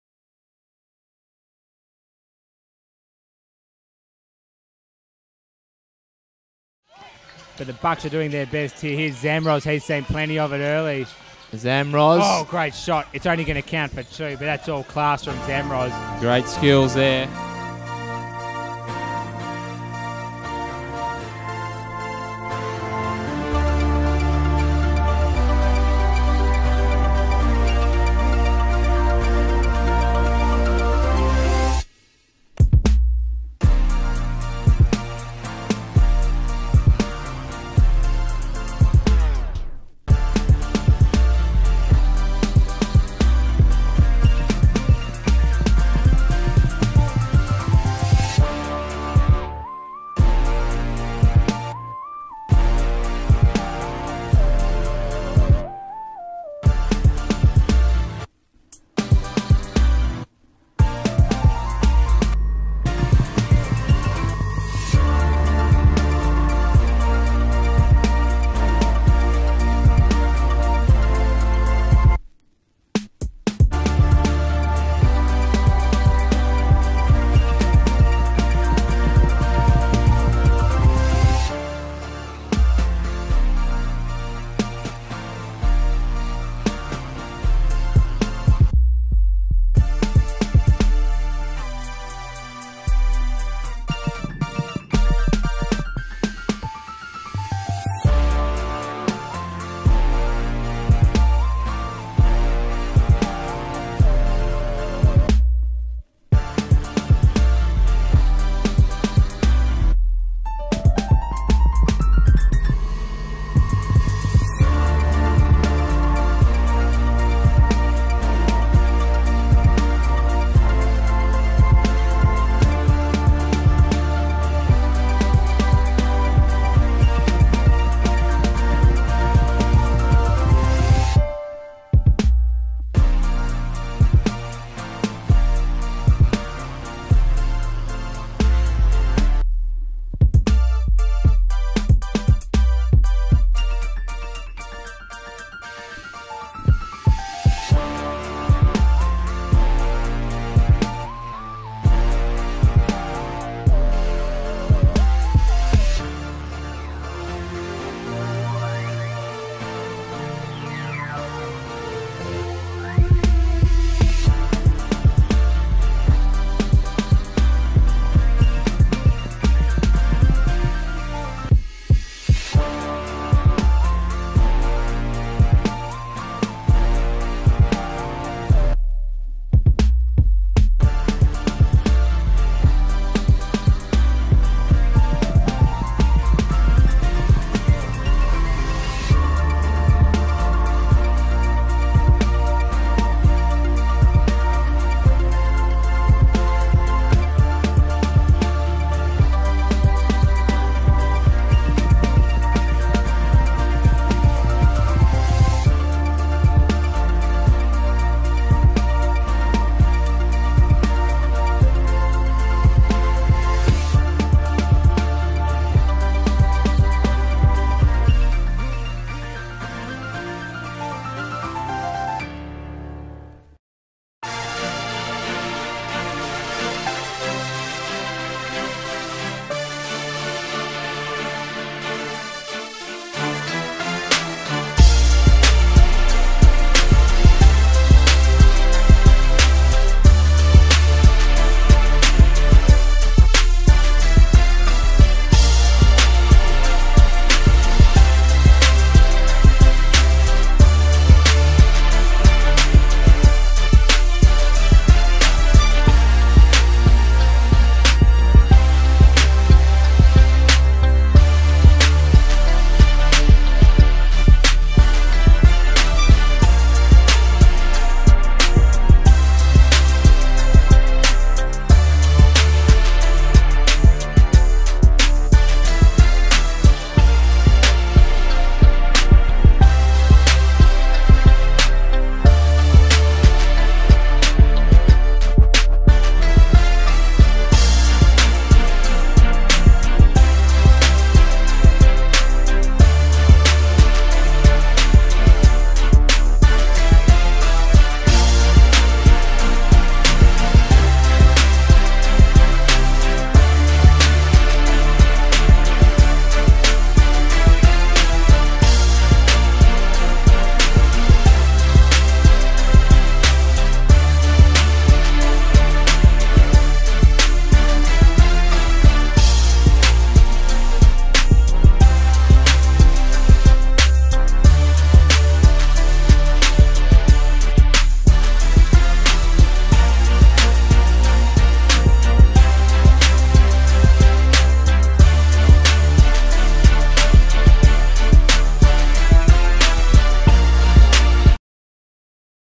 Zamroz SBL Highlights Geraldton Buccs
Ryan Zamroz - 6'4" Combo Guard - Bosman (Italian-American), NAIA All-American.  Highlights from Australia's State Basketball League.  #6 (#44 in some highlights). 31 ppg 5 apg 5 rpg